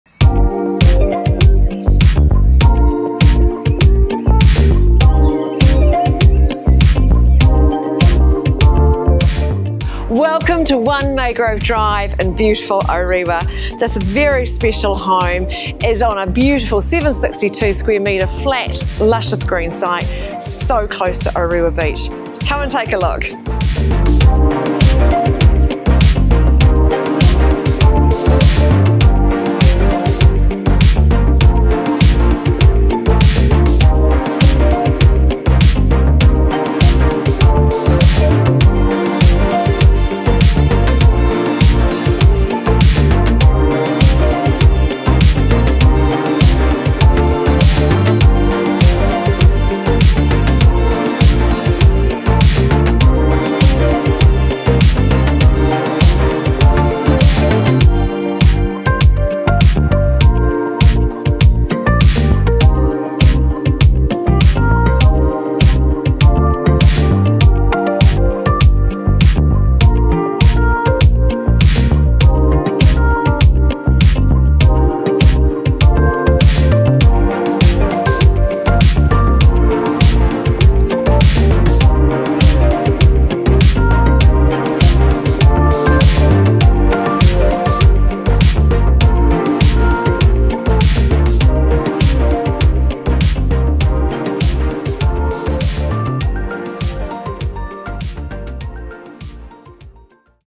1 Maygrove Drive Orewa
Welcome home to a residence that genuinely feels warm and welcoming, an oasis from life and a property you would be proud to call your own. This iconic landmark home nestled on a flat leafy green 762sqm flat site is in one of Orewas most coveted streets.
This quality home masterfully combines architectural sophistication with practical luxury and provides its occupants with resort-style living with its sunny aspect and heated inground pool.
Built in a time when construction was at its finest, certainty is here for the lucky new owners that the future is sound.
The large lawn easily accommodates a trampoline, space for children and pets, or can be developed into a garden.
At the heart of the home is a new, contemporary kitchen designed for modern life, showcasing the latest finishes and features.
Two generous living areas, each with a distinctive charm, open to the lush garden ideal for intimate gatherings and larger, lively entertaining.
The property has a user-friendly layout that provides many options, such as accommodating extended family or working from home, making the house a winner in the long term.
Located within a flat, easy walk to the beach, restaurants, and cafes, this home offers unparalleled convenience to local amenities, including schools.
The home's unique yet timeless design ensures it stands out while remaining practical and luxurious.
Experience Number One Maygrove Drive today - it's more than just a house; it's a place that truly feels like home.